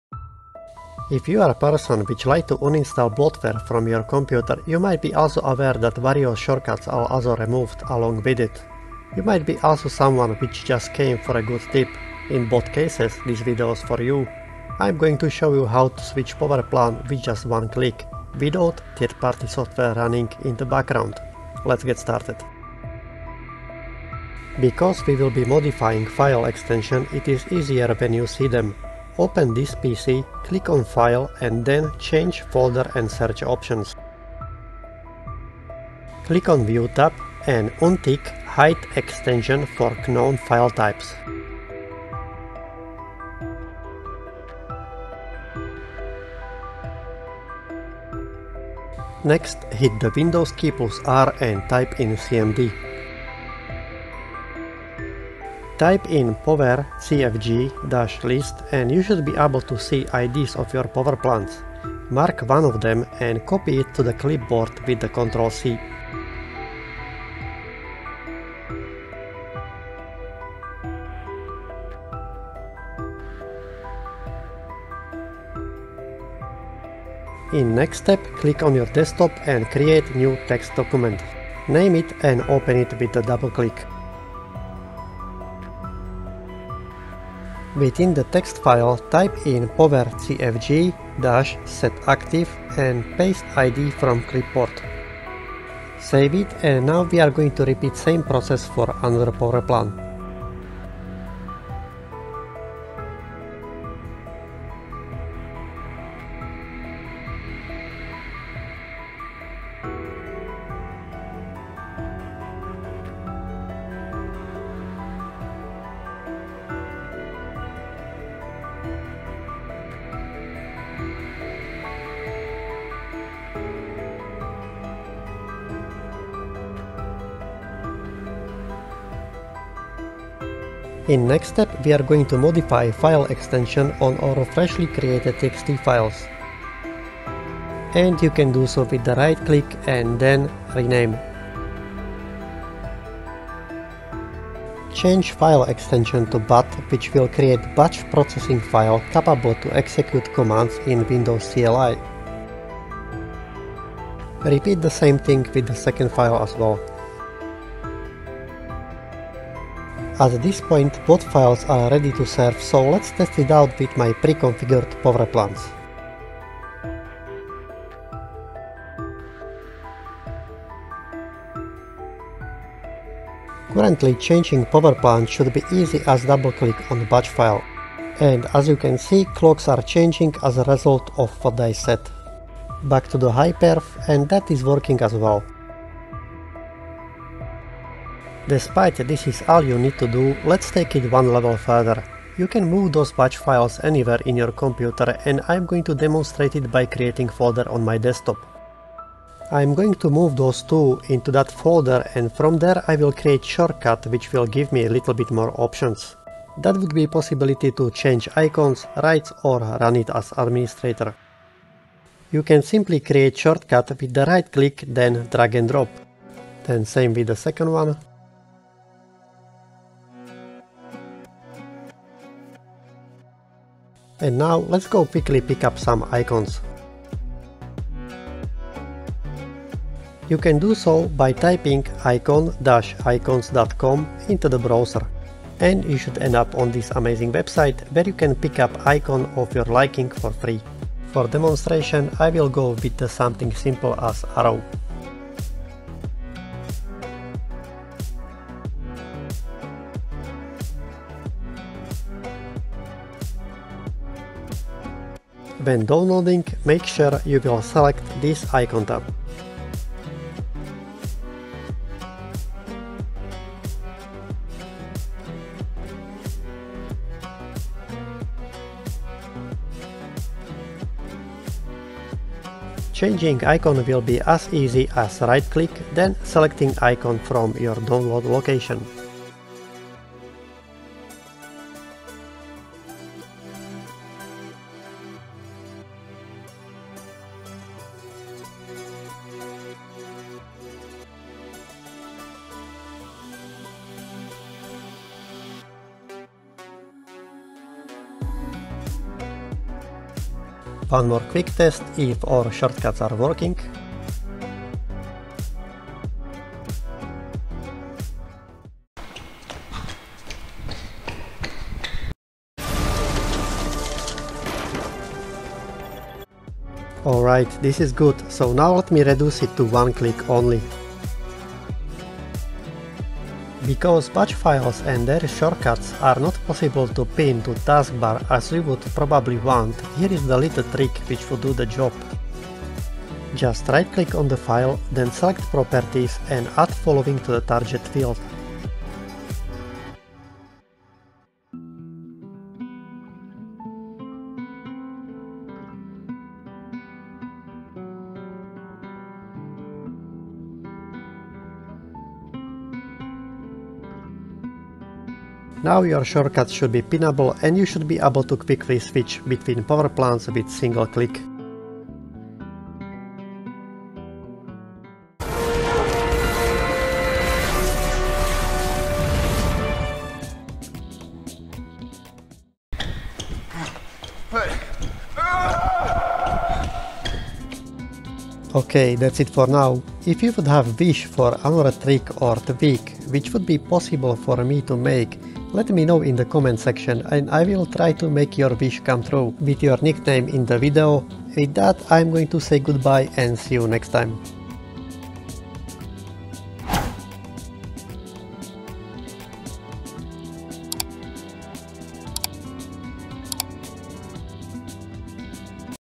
Expert Trick to SWAP Power Plans in Windows FAST 🚗
In this video, you'll learn Expert Trick to SWAP Power Plans in Windows FAST to optimize your computer's performance and energy usage. Whether you need maximum performance for intensive tasks or a power-saving mode for longer battery life, this guide will show you the fastest and easiest methods to change your power settings.

Should be applicable on most of the Windows versions.

Yours CMD Tech